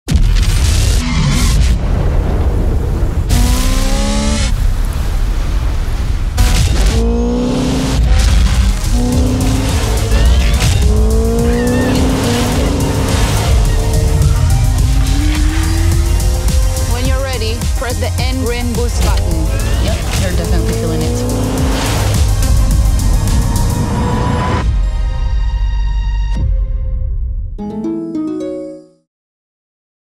Hyundai vám predstavuje Experiment N (30s)
Rjukan v Nórsku patrí k najtemnejším mestám v Európe. Chlad a nedostatok slnka má negatívny vplyv na duševnú pohodu miestnych ľudí. Môže mať však jazda s IONIQ 5 N na obyvateľov Rjukanu a jeho okolia pozitívny efekt? Dokáže im pomôcť prekonať to, čomu sa hovorí zimná depresia alebo splín? Vedci a Hyundai spojili sily, aby zmerali DOJO, čo je skratka pre Depth of Joy – mieru radosti. Pozrite sa, ako tento zaujímavý experiment prebiehal a ako to nakoniec celé dopadlo.